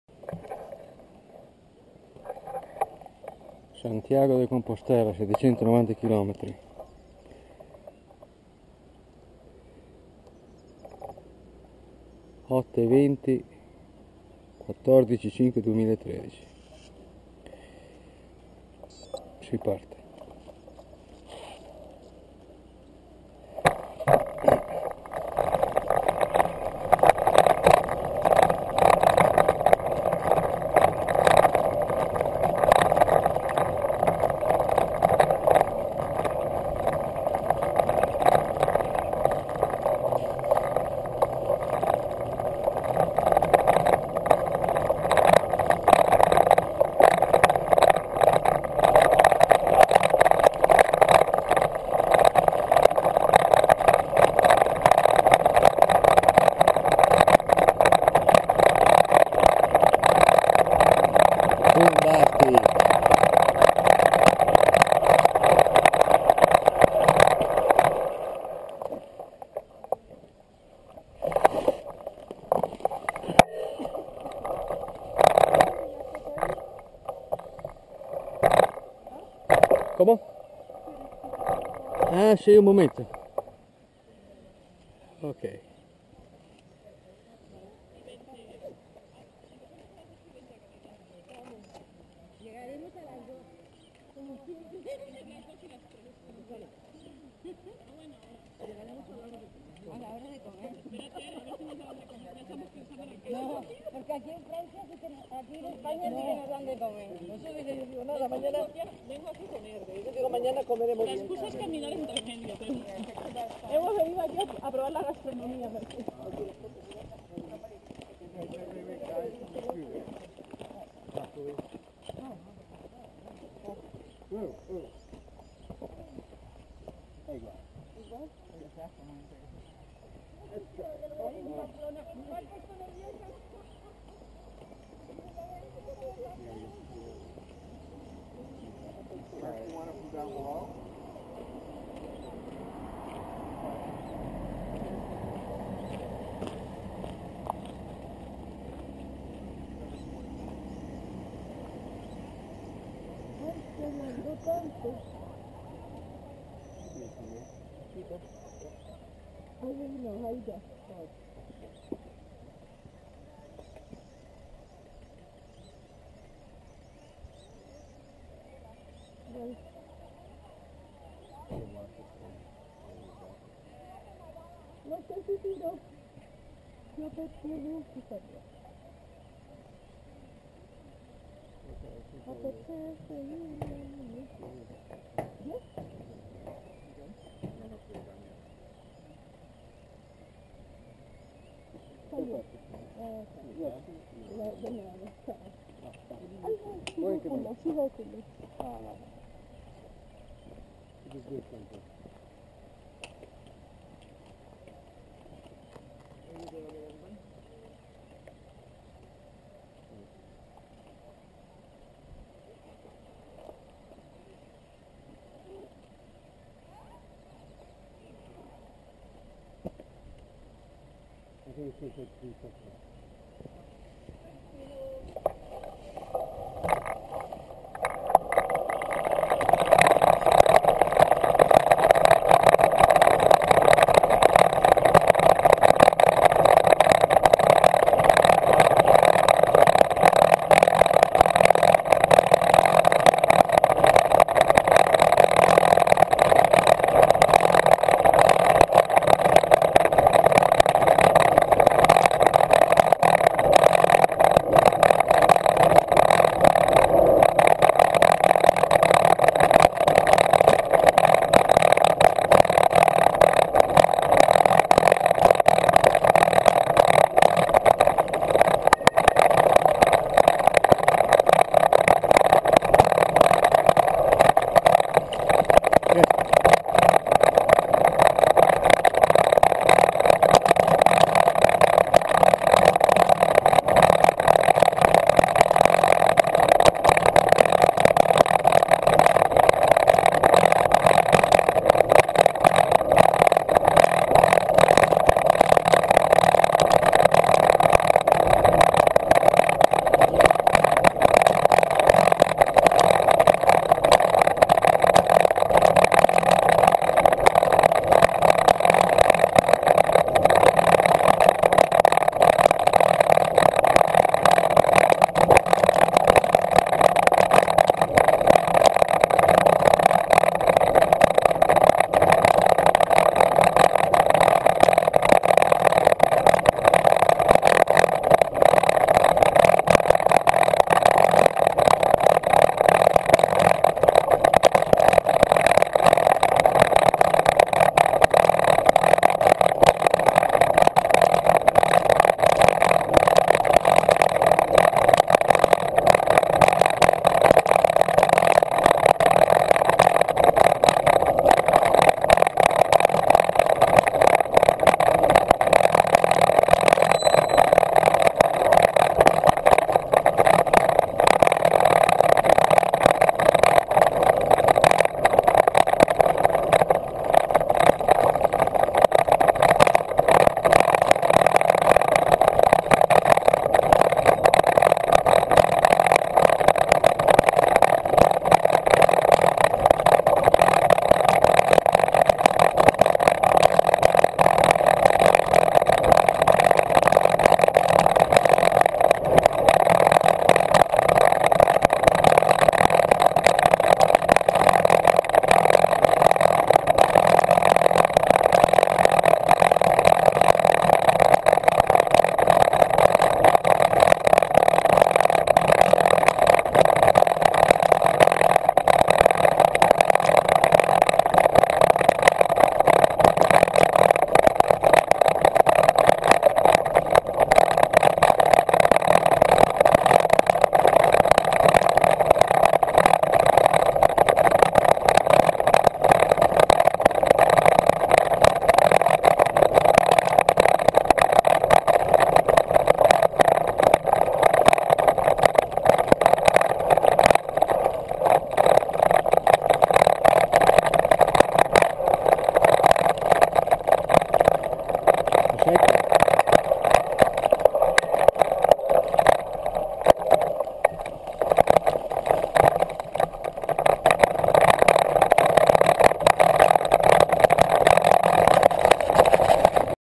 CAMINO DE SANTIAGO 1°ETAPA CLIP 01
PRIMO TENTATIVO DI PARTENZA E GRANDE INDECISIONE
CAMINO FRANCES